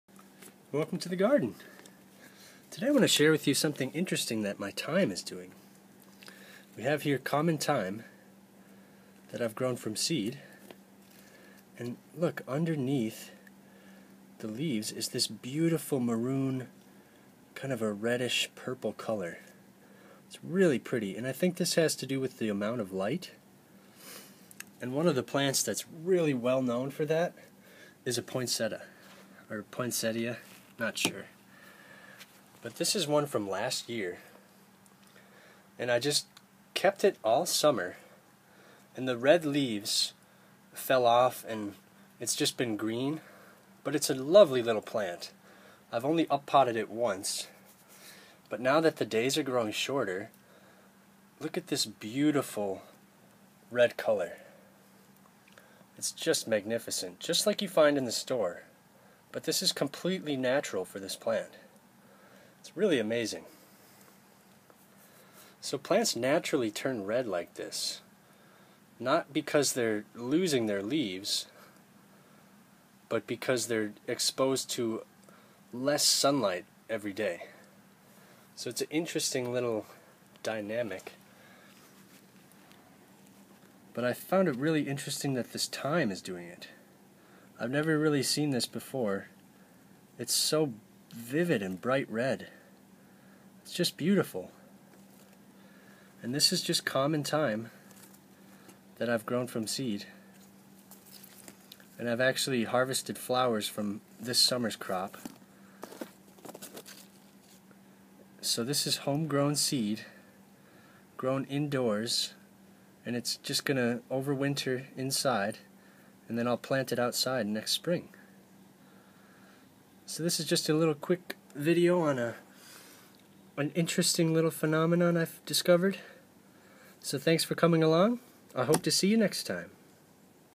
Red Thyme and Poinsettia
Poinsettia is a plant that's well known for its red leaves. The long dark nights trigger it to grow bright red foliage around the holiday season. This Poinsettia was grown indoors all summer where it had green leaves up until a few weeks ago. Thyme grown indoors in the winter months has surprised me with a similar display. The underside of the leaves has turned a redish color. This is a natural process that adds some extra color in the cold winter months.